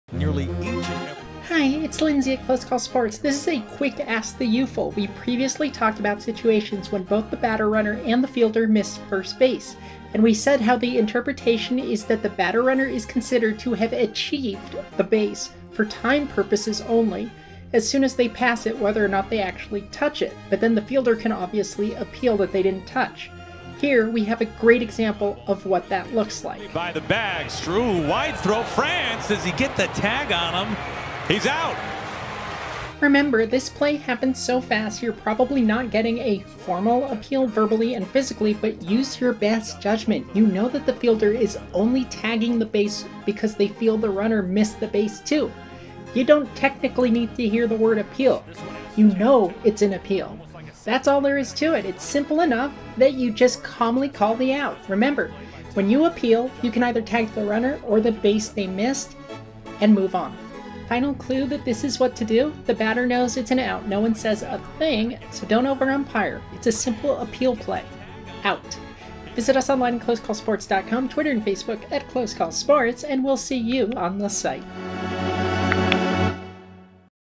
Ask UEFL - Batter-Runner & Fielder Both Missing 1st Base is a Common Appeal Play
In this Ask the UEFL, we take a closer look at 1B Umpire John Libka's out call in Seattle as both A's batter-runner Chad Pinder and Mariners first baseman Ty France missed first base. Article